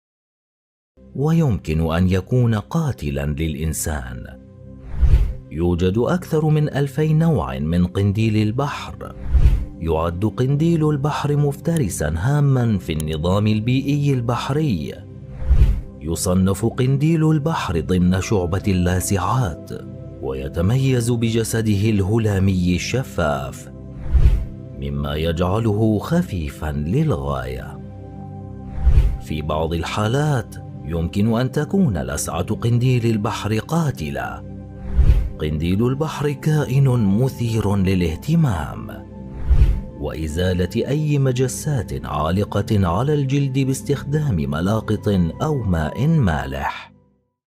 وثائقي قنديل البحر *jellyfish * سامر يونس
الصوت الفريد ،أخطر الكائنات البحرية ،  قنديل البحر ، لسعة قنديل البحر ، قناديل البحر ، وثائقي ، jellyfish

قنديل البحر أجمل الكائنات البحرية وأخطرها (jellyfish) تتميز بأنواعها العديدة